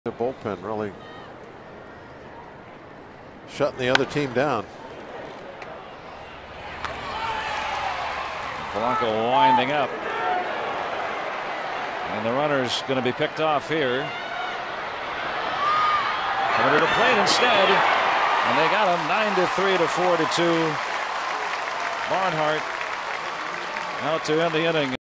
CIN@PIT: Pirates turn two to end the 11th
Gregory Polanco catches Ivan De Jesus' fly ball and starts a relay that catches Tucker Barnhart in a rundown for the final out of the frame